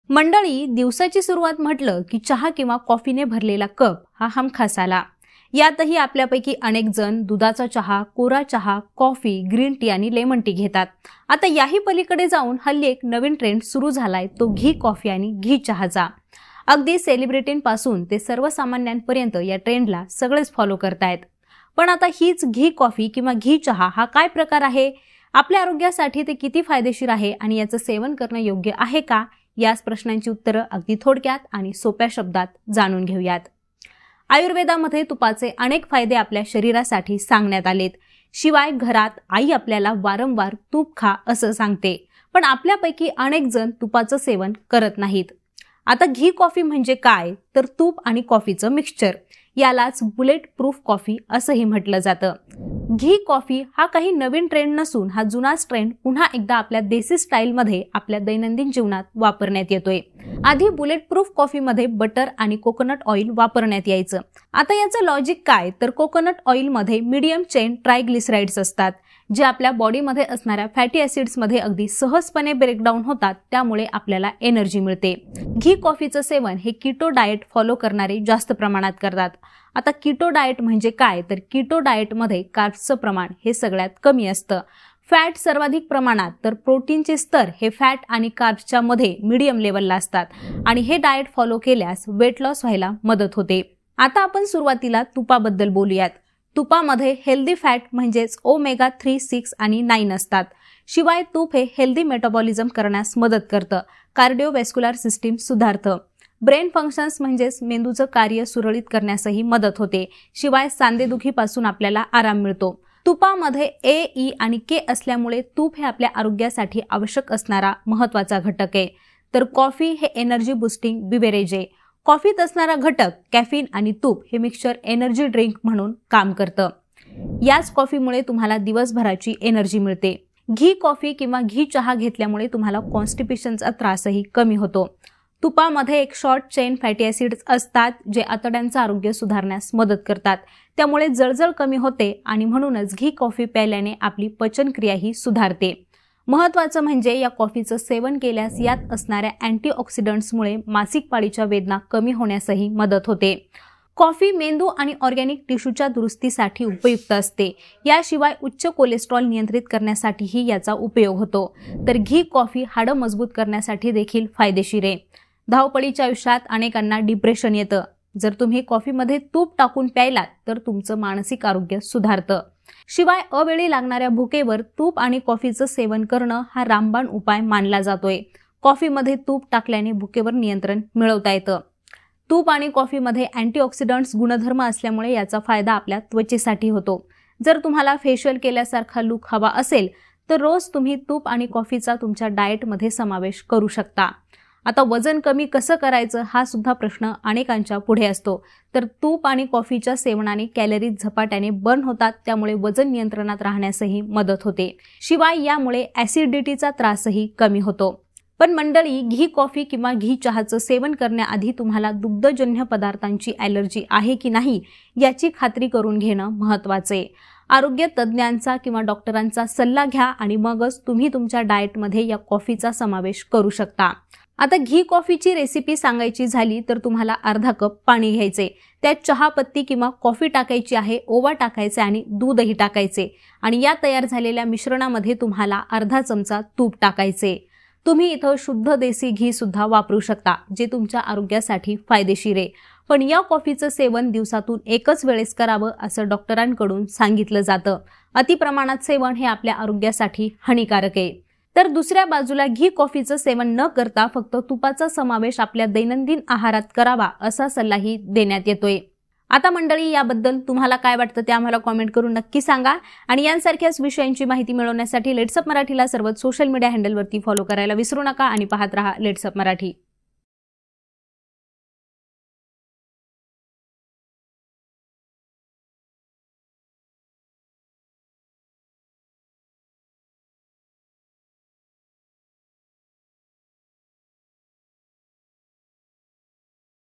Healthy Life Style : घी कॉफी अन् घी चहाचे फायदे-तोटे... सोप्या भाषेत समजून घ्या! | LetsUpp Marathi
घी कॉफी आणि घी चहा प्रकार आहे तरी काय? याचे फायदे-तोटे काय आहेत ते सोप्या भाषेत समजून घ्या!